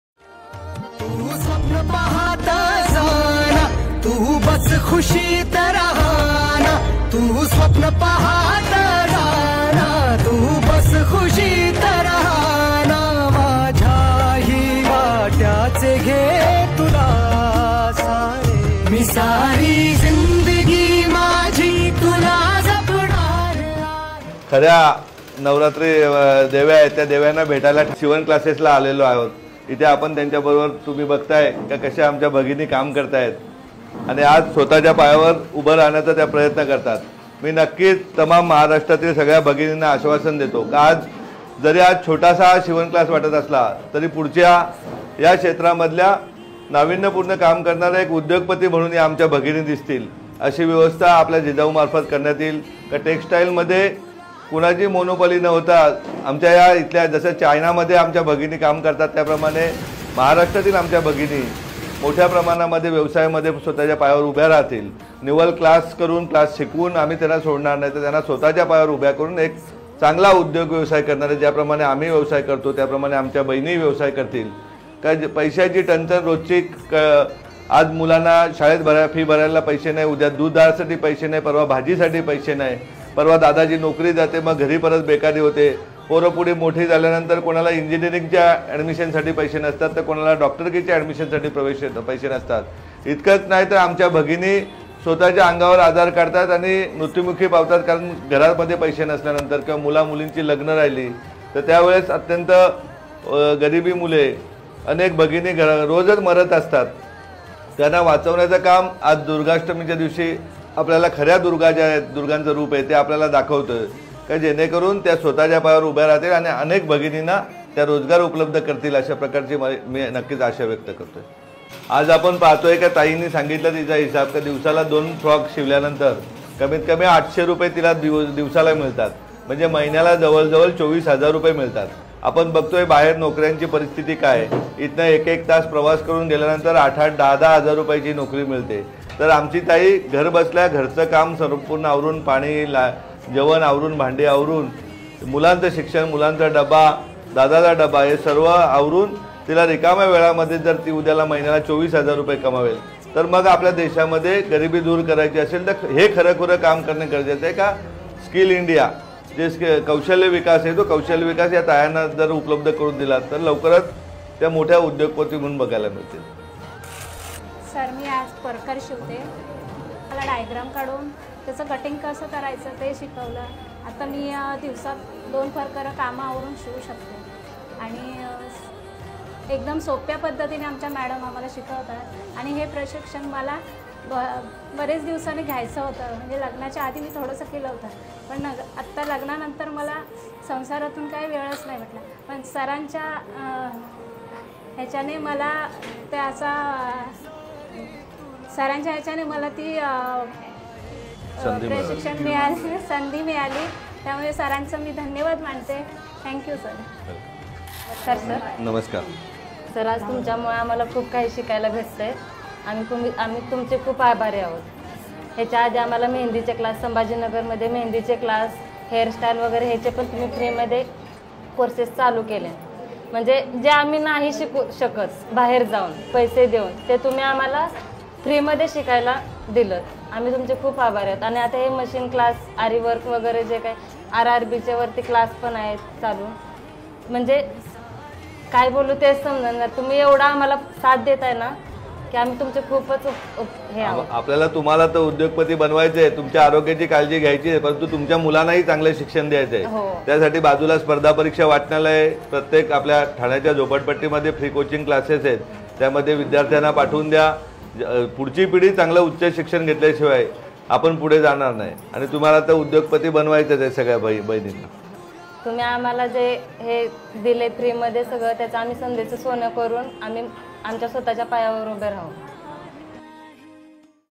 जिजाऊ संस्थेचे महिलांसाठी मोफत शिलाई प्रशिक्षण उपक्रम !!!
कोकणातील पाचही जिल्ह्यात जिजाऊच्या शिलाई प्रशिक्षण व मॉर्डन फॅशन क्लासेसला महिलांचा उस्फूर्त प्रतिसाद...!!